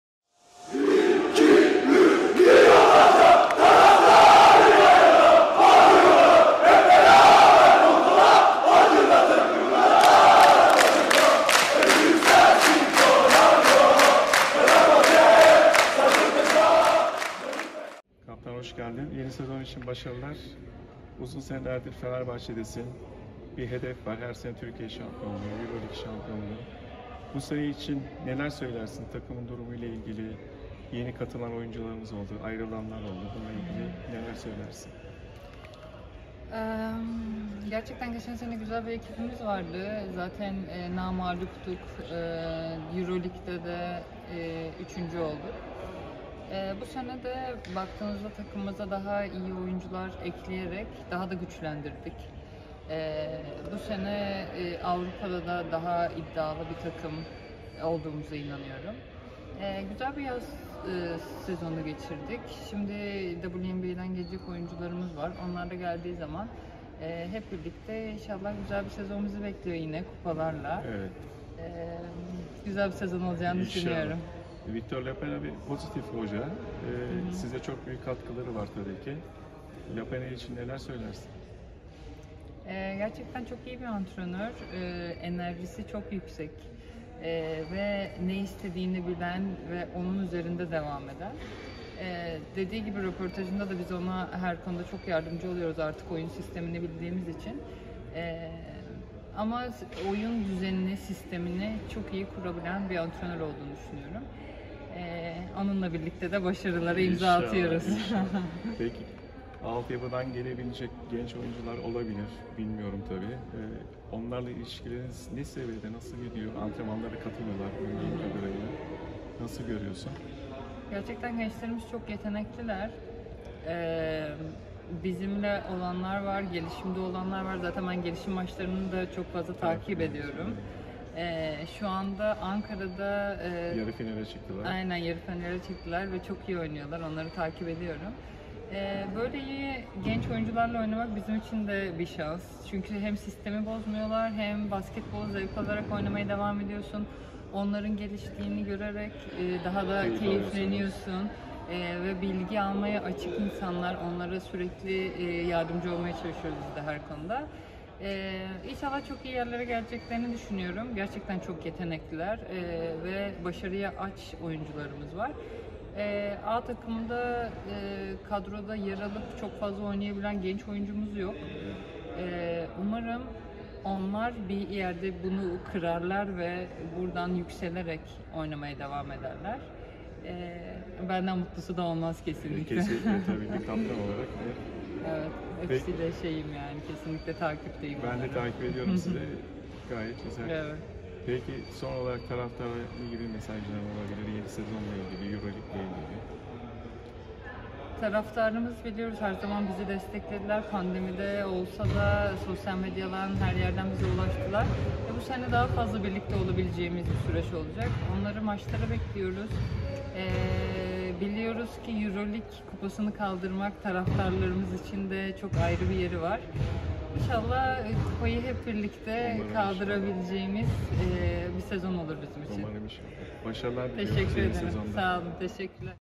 Salon Tribünü #53 - Konuğumuz Tuğçe Canıtez
2013'ten beri formasını giydiği Fenerbahçe Kadın Basketbol Takımı'nın kaptanlığını yapan milli oyuncumuz Tuğçe Canıtez, yeni sezon öncesi düzenlenen Medya Günü etkinliğinde Salon Tribünü ekibinden Aydın Şenyuva'nın sorularını yanıtladı.

Fenerbahçe basketboluna tribünden, amatör ruhla bakış. Podcast'ler ve konuşulan konularla alakalı her türlü soru ve görüşünüz için Twitter'dan ulaşabilirsiniz.